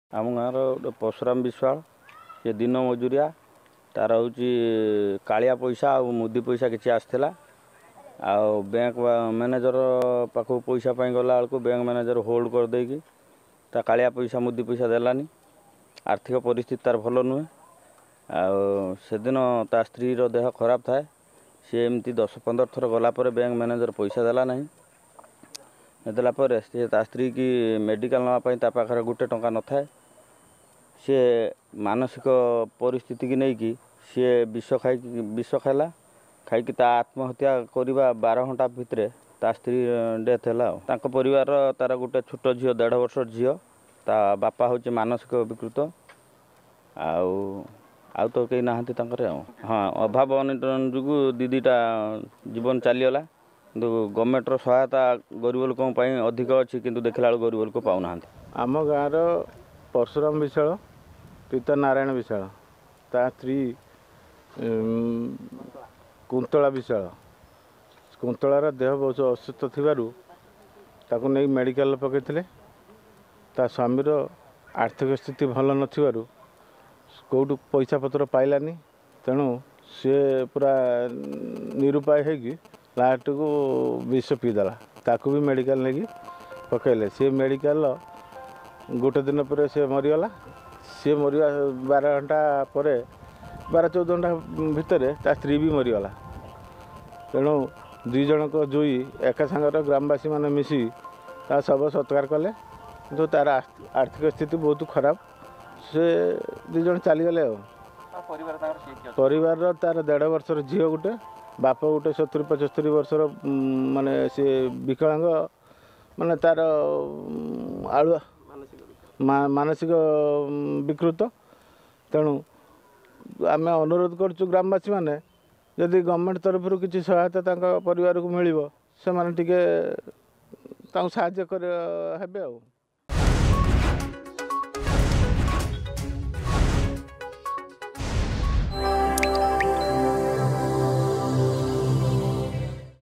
ଏକା ସାଙ୍ଗରେ ଜଳିଲା ଦମ୍ପତ୍ତିଙ୍କ ଯୁଇ
ବାଲେଶ୍ୱର ଜିଲ୍ଲା ସିମୁଳିଆ ଥାନା ଅଡା ପଞ୍ଚାୟତ ଅଧିନ ହଳିସୋ ଗାଁରେ ଦେଖିବାକୁ ମିଳିଛି ଏକ ହୃଦୟ ବିଦାରକ ଦୃଶ୍ୟ । ଏକା ସାଙ୍ଗରେ ଜଳିଲା ଦମ୍ପତ୍ତିଙ୍କ ଯୁଇ l ଏହି ଗାଁର ନାରାୟଣ ବିଶ୍ୱାଳ ଜଣେ ମାନସିକ ବିକାରଗ୍ରସ୍ତ ହୋଇଥିବା ବେଳେ ତାଙ୍କର ପୁଅ ପର୍ଶୁରାମ ଓ ବୋହୂ କବିତାଙ୍କର ଗୋଟିଏ ଦିନରେ ଚାଲିଯାଇଛି ଜୀବନ